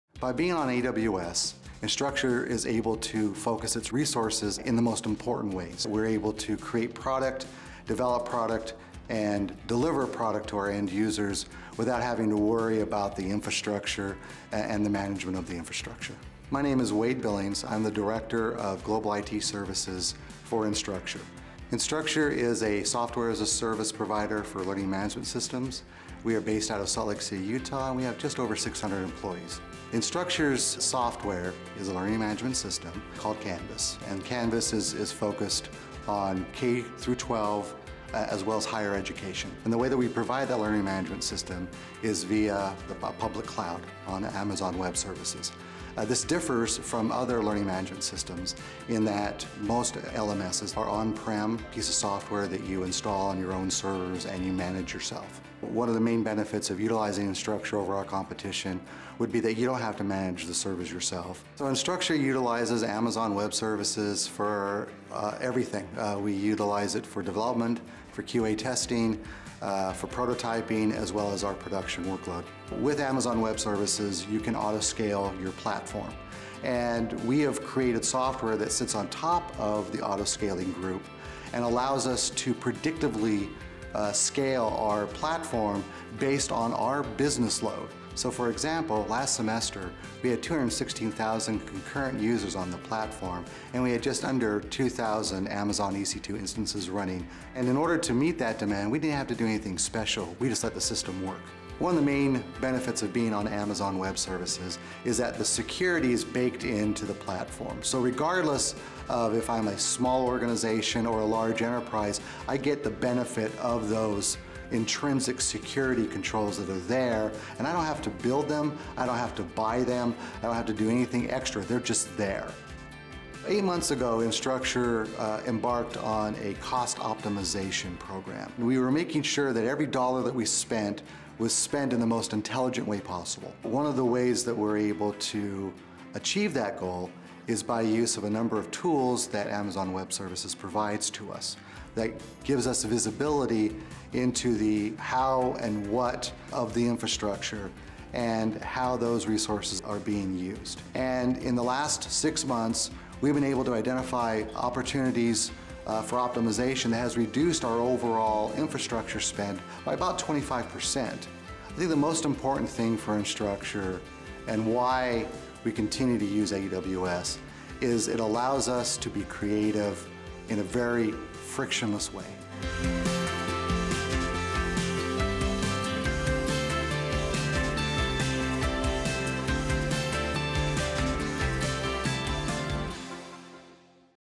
Instructure Delivers Online Learning Globally Using AWS
Instructure is a Salt Lake City–based company that provides an online learning management system (LMS) used by more than 1,200 colleges, universities, and school districts around the world. Unlike most LMS products that are installed locally, Instructure decided to run all development, testing, and production workloads in the AWS cloud. Instructure benefits from the scalability of AWS, successfully handling more than 200,000 concurrent users when demand spikes while protecting the confidentiality of its customers with the built-in security of the AWS platform.